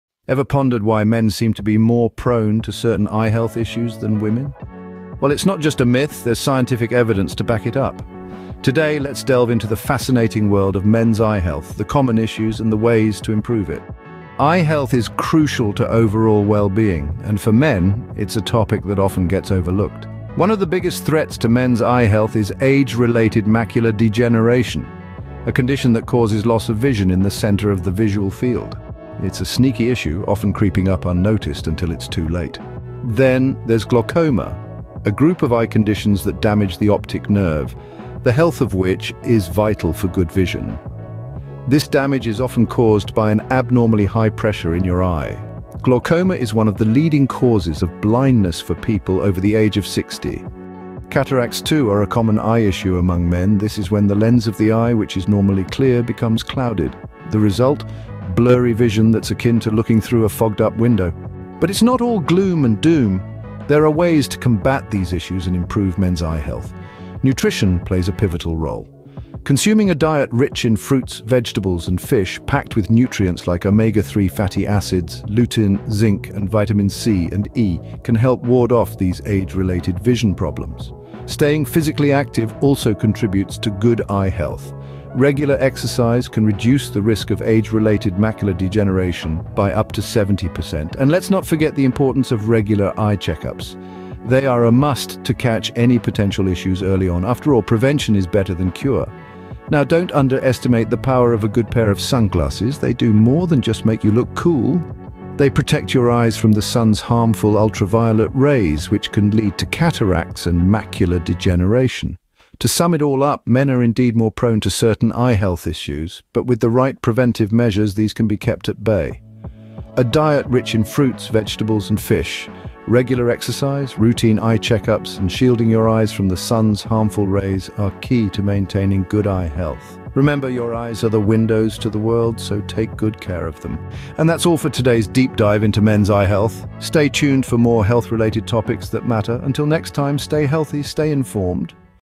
Eyes Wide Open A Guide to Men's Eye Health
In this video, we discuss men's eye health and steps to help prevent issues.